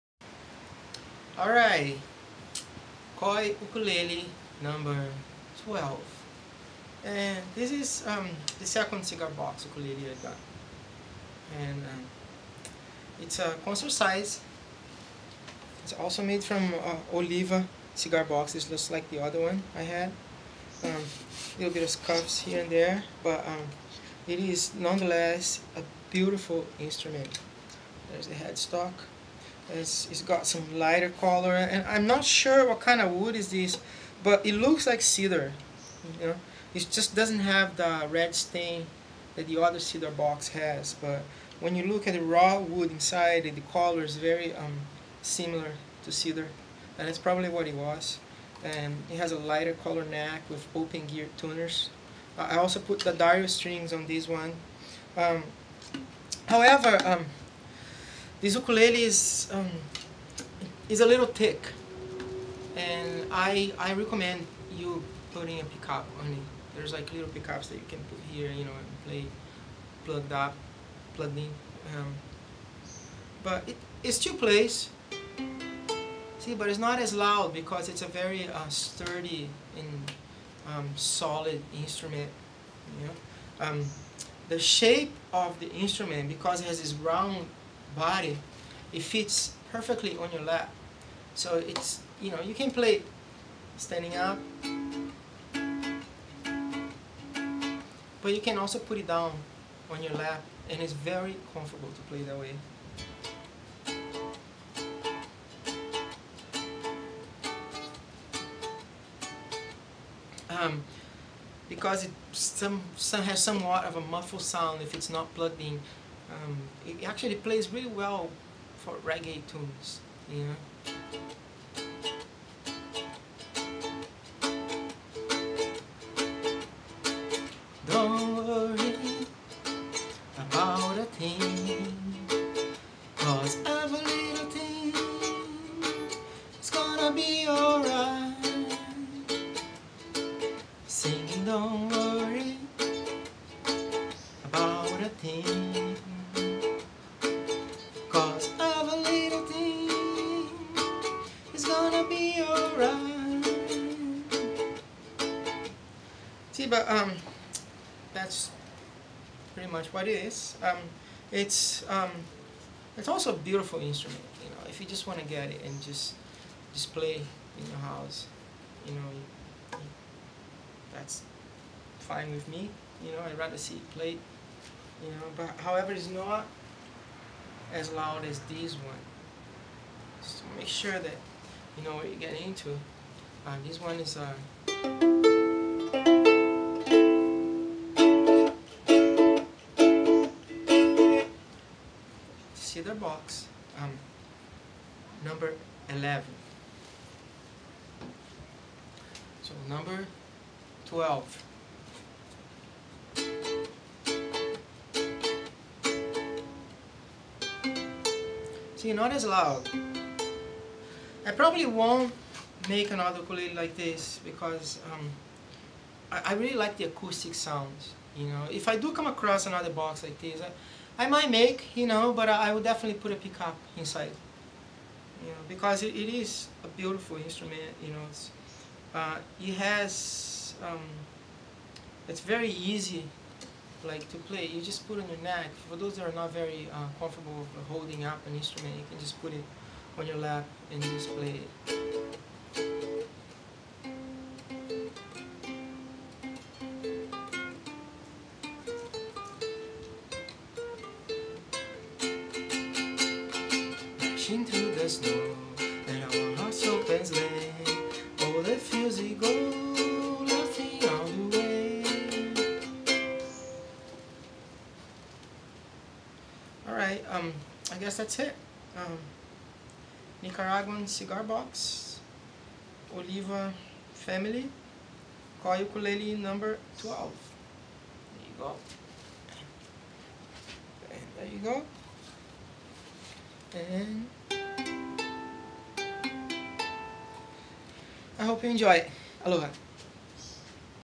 Koi Ukulele #12 Cigar Box
So this is my second cigar box uke. The top is a little thick, muffling the sound a bit. This would go great with a pick up. It is my most beautiful instrument. It is also super comfortable to play sitting down, the uke just rests perfectly in your lap, due to its round body. Concert size neck, with open geared tuners. No cigar box uke is equal, so if you like this one, get it before someone else, because, yes, it is for sale. Aloha, Leo!